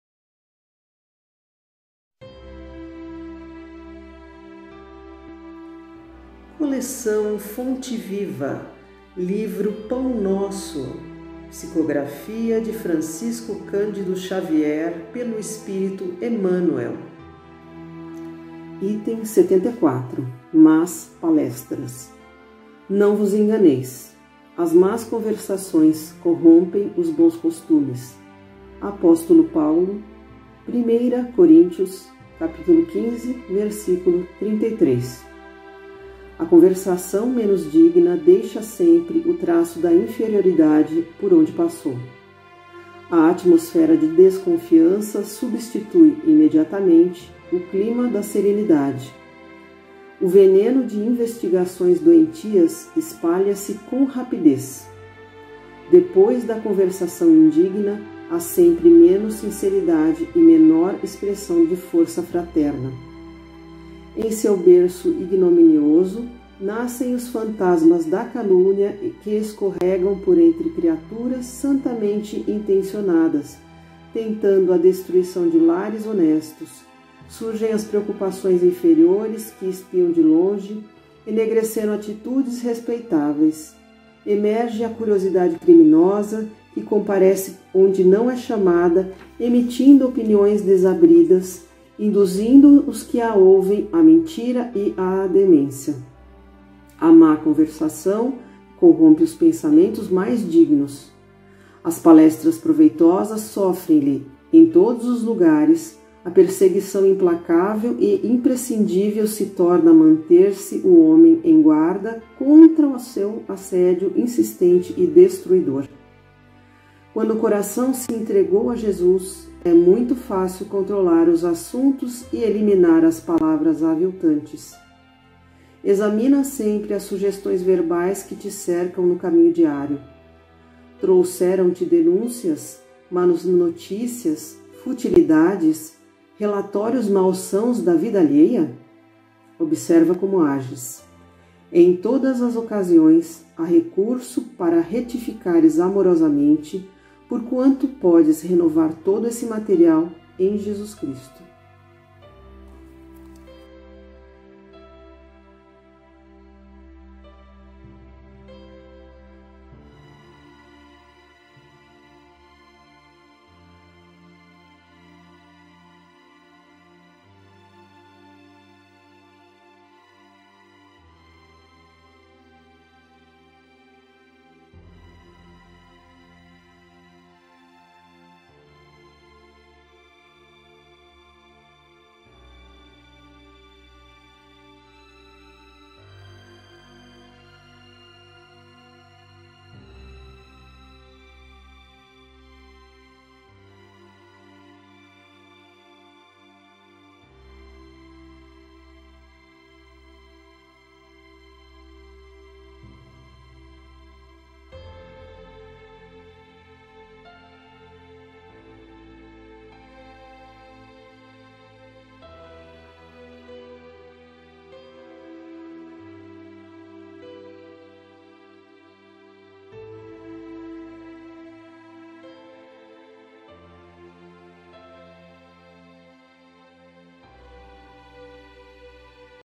Livro "Pão Nosso"-Item 74: Más palestras #021
A Sociedade Espírita Ponto de Luz - Rolante/RS realiza a leitura da "Coleção Fonte Viva" (psicografia de Francisco Cândido Xavier, pelo Espírito Emmanuel; Editora FEB).
Item 74: Más palestras - Livro Pão Nosso #021